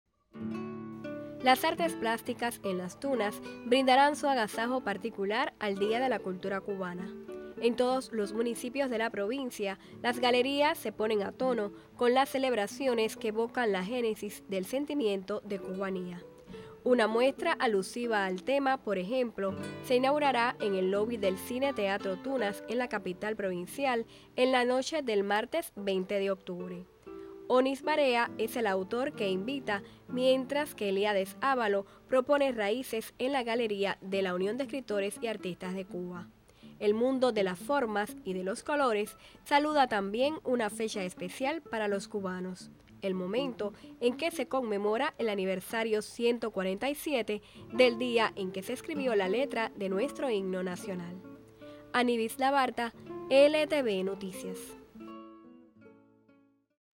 Las Artes Plásticas tuneras presentes en el Día de la Cultura cubana
anybis Labarta con los detalles...